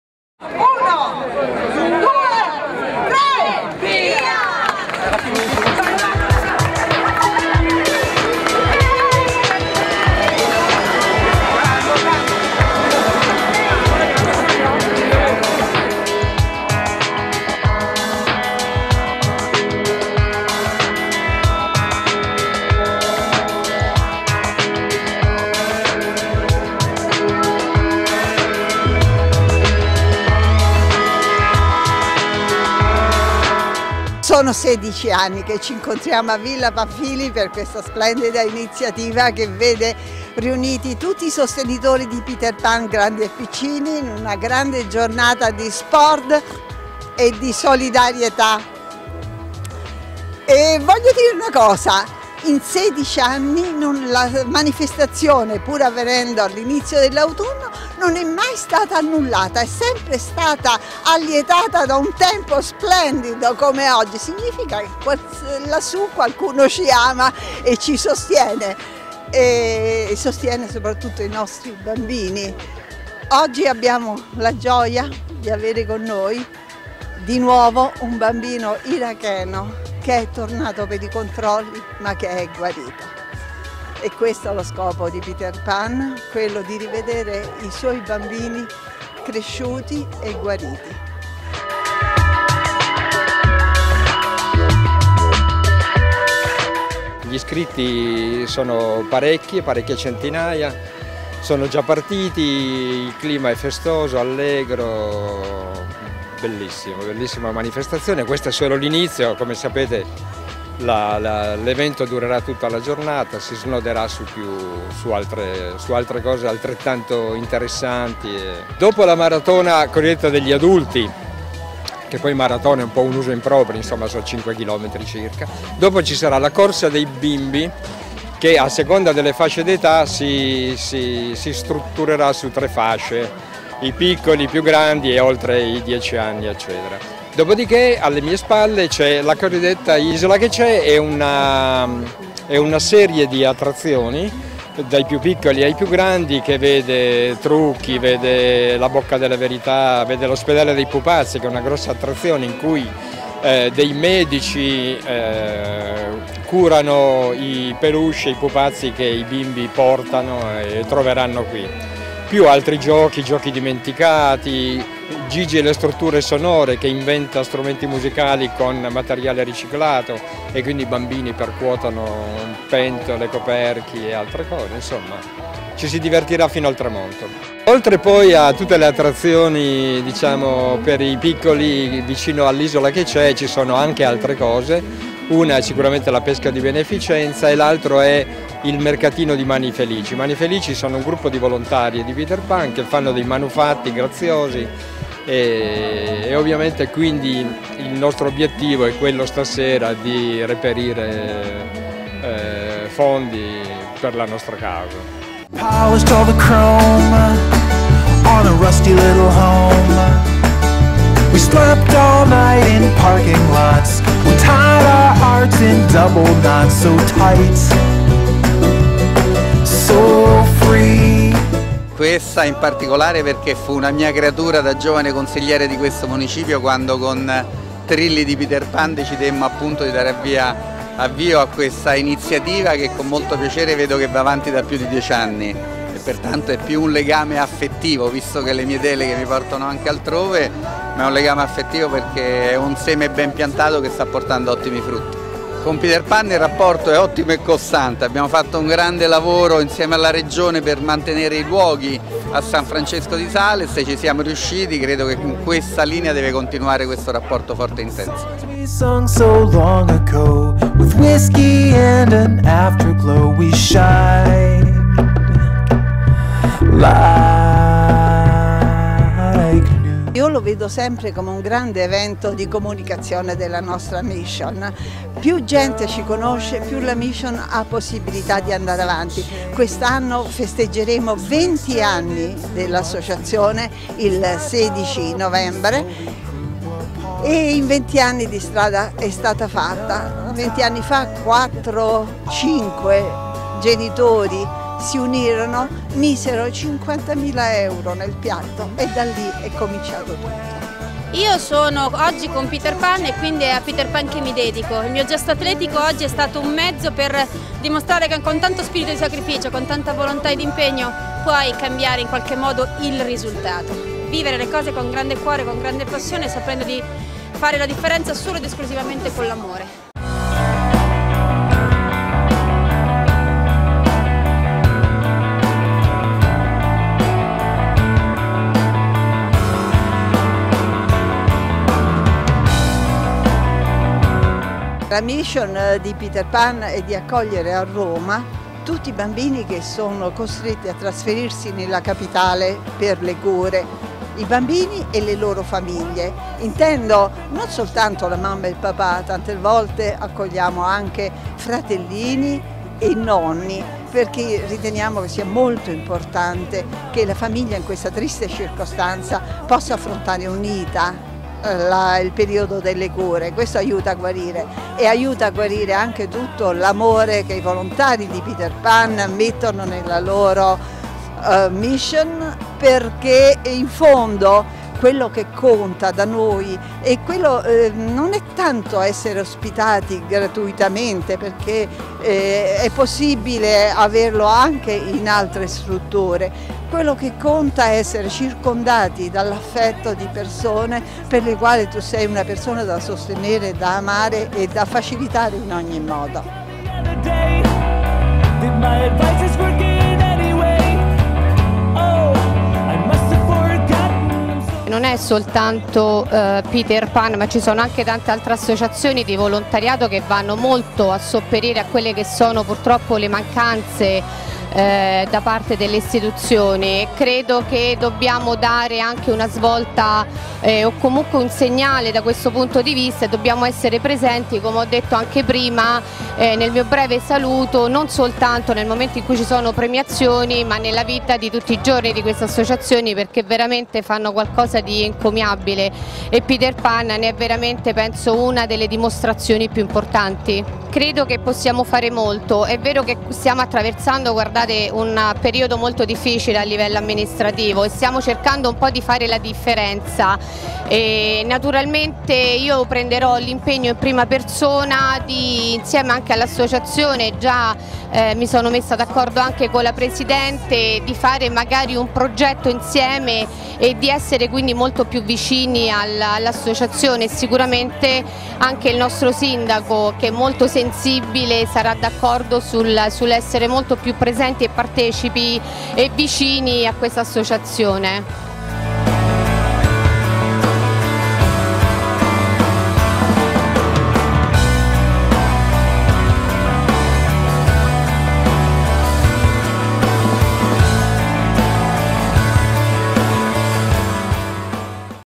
XVI edizione della Maratona di Peter Pan
Servizio trasmesso dall'emittente Rete Oro sulla XVI edizione della Maratona di Peter Pan, organizzata dall'associazione omonima domenica 28 settembre presso Villa Doria Pamphili a Roma.
Tra i partecipanti, la campionessa paralimpica Annalisa Minetti. Presenti anche l'assessore ai Lavori Pubblici di Roma Capitale Paolo Masini, la Presidente della Commissione delle Elette dell'Assemblea Capitolina Daniela Tiburzi e l'attrice Irene Ferri.